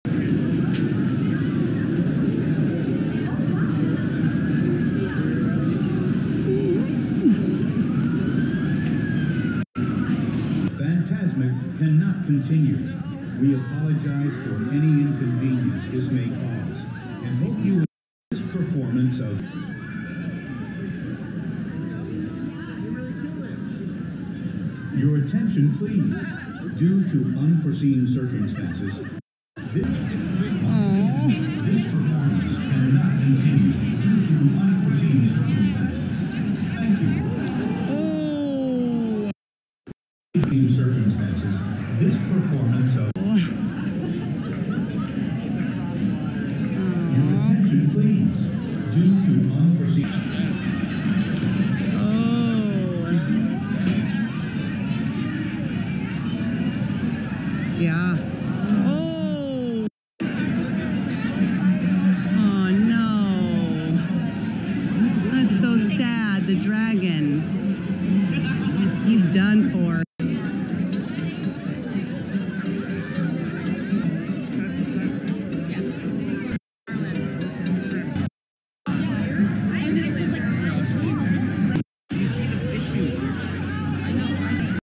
Fantasmic Le dragon de Disneyland prend feu
Dragon Catches Fire At Disneyland

A fire broke out at Disneyland during the Fantasmic ! nighttime show with the Maleficent dragon catching flames.

Disneyland theme park representatives confirmed that “during the final showing of Fantasmic at Disneyland park on Saturday evening, the dragon caught fire. Anaheim Fire & Rescue quickly responded and the fire was extinguished.”